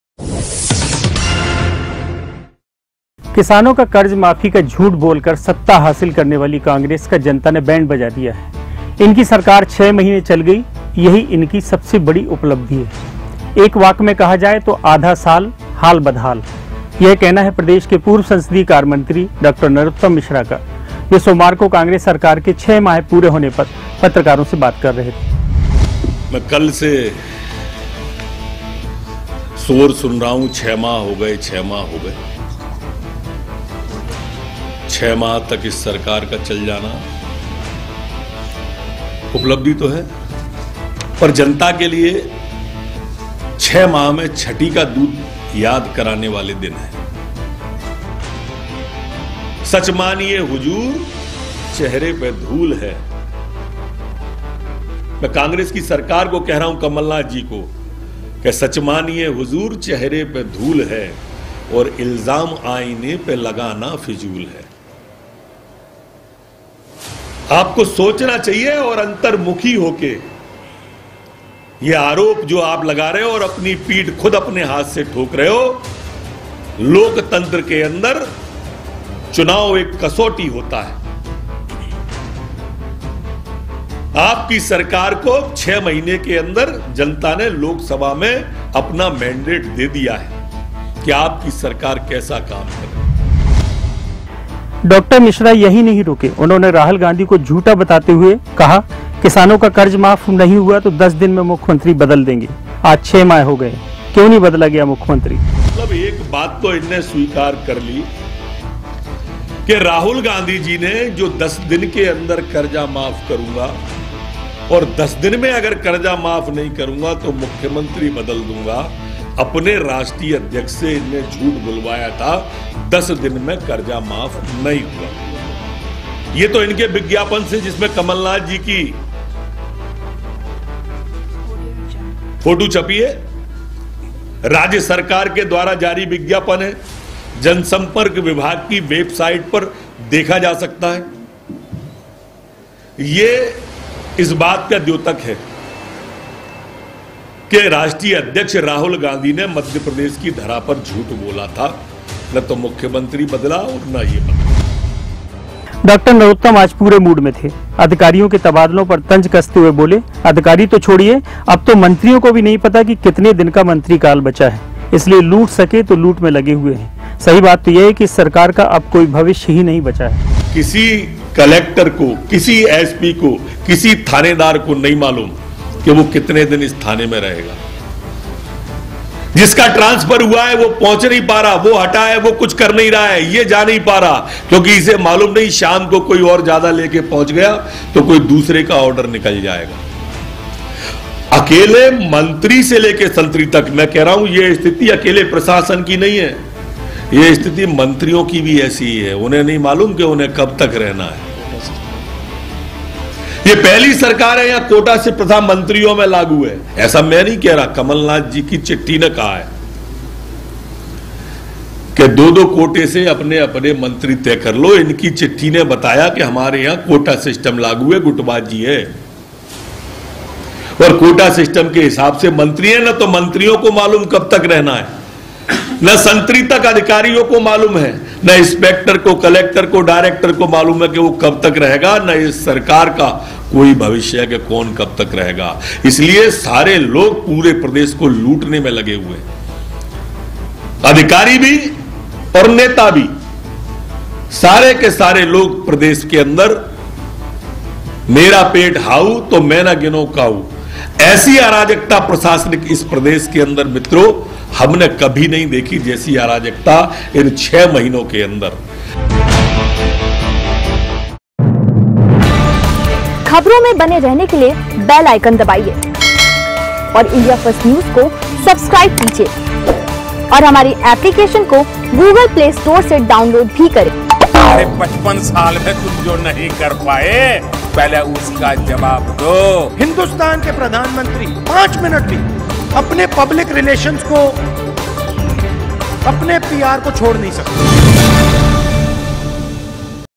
NAROTTAM MISHRA . ये बेमिसाल नही बदहाल !!
6 महीने की सरकार पर गरजे डाॅ नरोत्तम
आधा साल... बेमिसाल नहीं बदहाल.
किसानां की कर्जमाफी का झूठ बोलकर सत्ता हासिल करने वाली कांग्रेस का जनता ने बैंड बजा दिया है... इनकी सरकार 6 महीने चल गयी यही इनकी सबसे बड़ी उपलब्धि है. एक वाक्य में कहा जाए तो आधा साल हाल बदहाल. यह कहना है प्रदेश के पूर्व संसदीय कार्यमंत्री डाॅ नरोत्तम मिश्रा का, वे सोमवार को कांग्रेस सरकार के छह माह पूरे होने पर पत्रकारों से बात कर रहे थे।
डाॅ मिश्रा यहीं नहीं रुके उन्होंने राहुल गांधी को झूठा बताते हुए कि उन्होंने कहा था, किसानों का कर्ज माफ नहीं हुआ तो 10 दिन में मुख्यमंत्री बदल देंगे, आज छह माह हो गए. क्यों नहीं बदला गया मुख्यमंत्री.
डाॅ नरोत्तम आज पूरे मूड में थे, अधिकारियों के तबादलों पर तंज कसते हुए बोले... अधिकारी तो छोड़िए.. अब तो मंत्रियों को भी नहीं पता कि कितने दिन का मंत्री काल बचा है... इसलिए लूट सके तो लूट में लगे हुए हैं। सही बात तो यह है कि इस सरकार का अब कोई भविष्य ही नहीं बचा है.
डाॅ नरोत्तम के वार पर वार
राहुल गांधी झूठे हैं.
क्यों नहीं बदला अब तक सीएम..
मंत्रियों को नहीं पता अपना भविष्य
जनता ने बजा दिया कांग्रेस का बाजा
नाकामियों का इल्जाम हम पर क्यों?
हमने गलत किया तो कार्यवाही करो
किसानों नौजवानों को पहनाई टोपी
छठी का दूध याद दिला रही है जनता